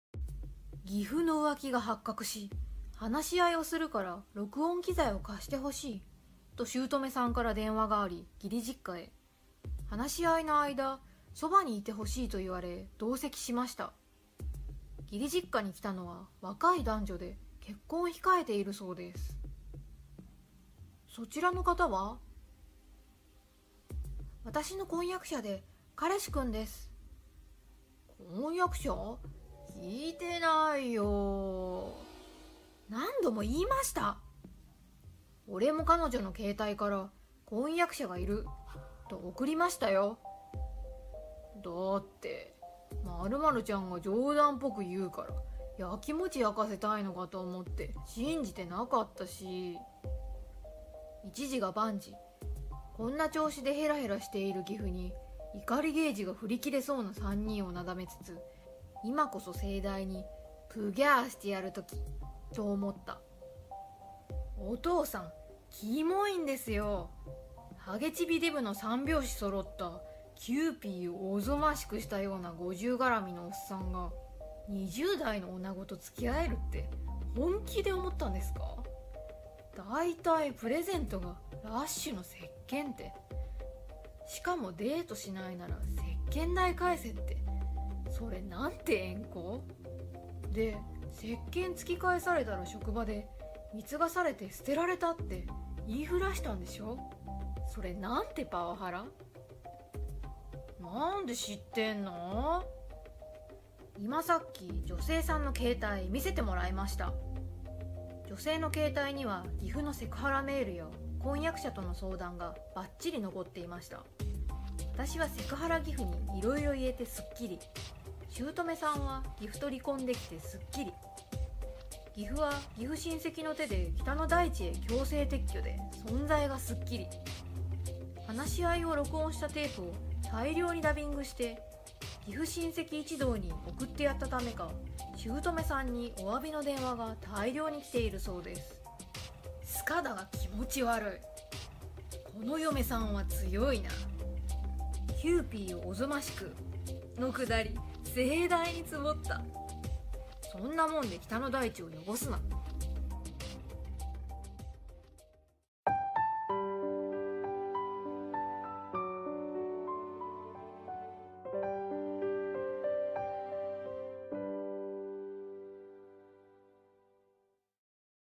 【スカッとする話】義父が若い女性と浮気したらしく、録音機材を持って義実家へ行ったら
皆様からの感想やコメントをお待ちしております。